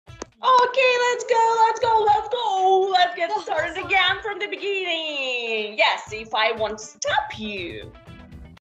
When I Turned on more energy to wake up my student)))
Actually there are many different ways to make student to pay attention on something when they lost it, distracted or just tired because of different reasons. And various changing of your way to talk is one of it. Start talking louder than before, or opposite, quieter. Diversity is one of the way to make your lessons more interesting and useful. Which ways do you use as a teacher to make this process funnier and useful at the same time?